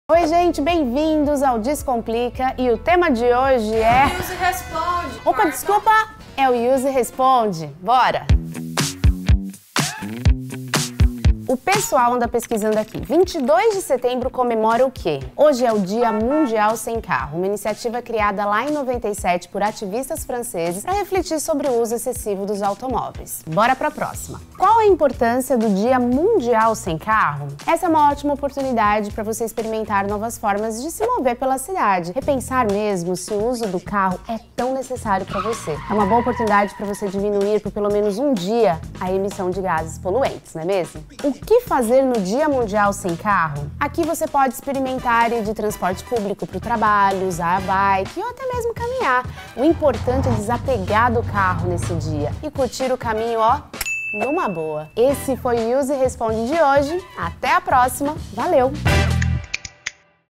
Youse Responde: Dia Mundial Sem Carro
Hoje é o Dia Mundial Sem Carro e você sabe o que essa data representa? Se a resposta for não, você está no lugar certo. Confira as respostas das principais dúvidas sobre o tema. Tem mais alguma? Manda pra gente nos comentários. Aqui é assim: vocês perguntam e a Youse Responde. Dá o play!